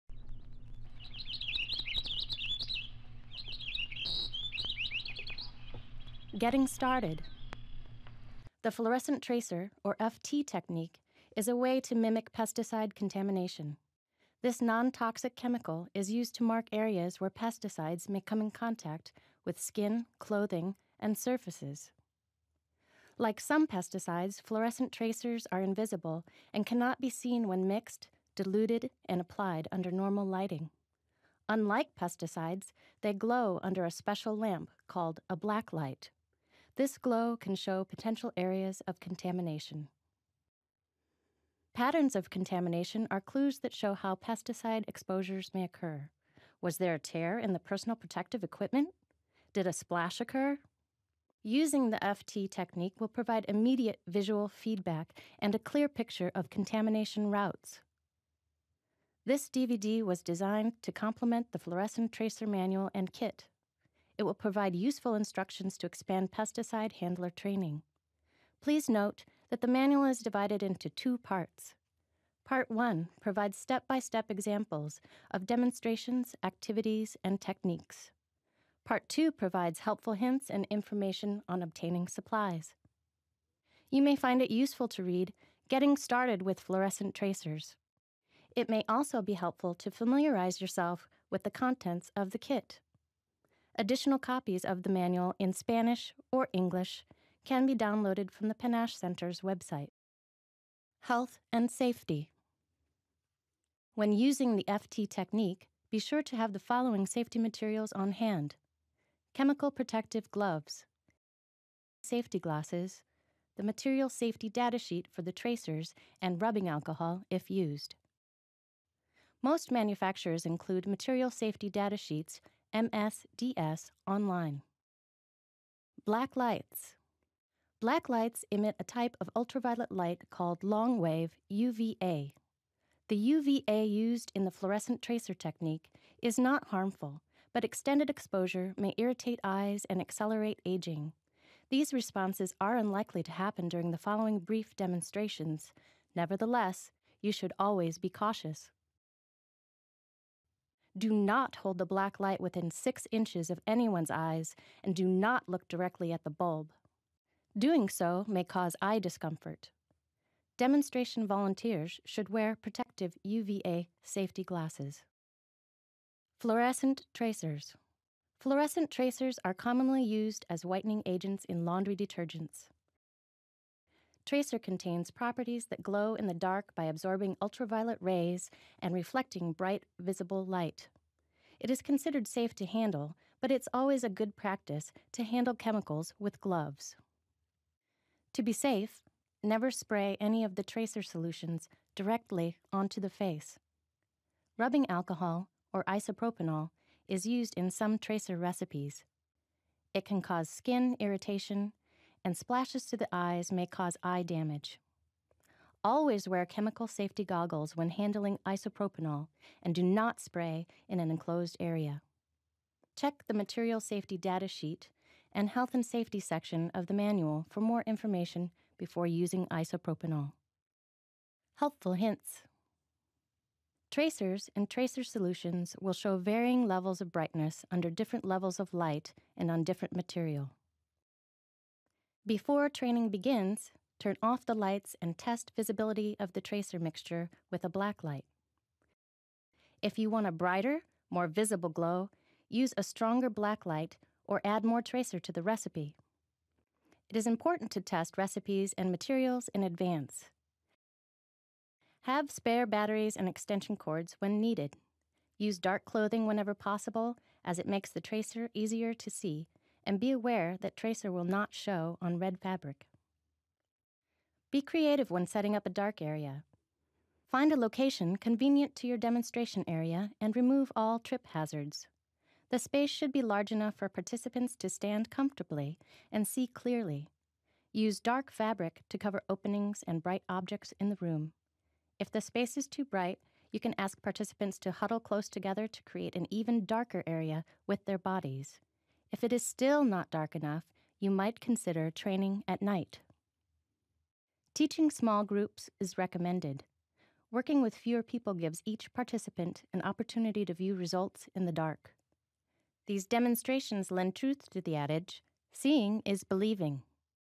Fluorescent Tracer 2 of 6: Getting Started
This video was developed to assist pesticide safety educator training and should be used as a companion to the Fluorescent Tracer Manual and Kit. It contains instruction and tips for preparation of materials, training areas, and dark rooms. This video walks the viewer through lessons learned during quick demonstrations, and actual airblast applications. This video is great for instructors and applicators alike.

This video was produced by the PACIFIC NORTHWEST AGRICULTURAL SAFETY AND HEALTH (PNASH) CENTER at the University of Washington with funding from NIOSH/CDC #2 U50 OH07544 and the Washington State Medical Aid and Accident Fund.